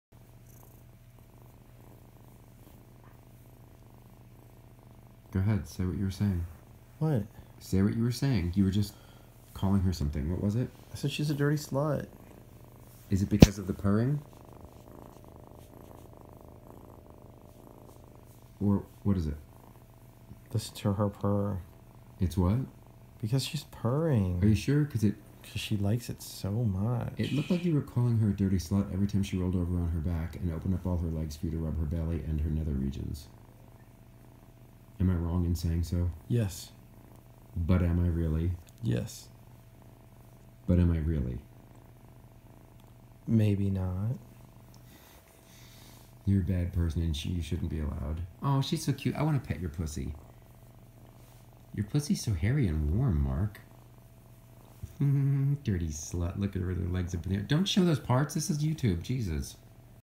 FurryFriday 7/20/2018
Yet another UglyKitten for this weeks FurryFriday. You want to turn up the volume if you want to hear her purring. But you want to turn down the volume if you don’t want to hear the PottyMouth conversation between myself and the pervert who is petting her.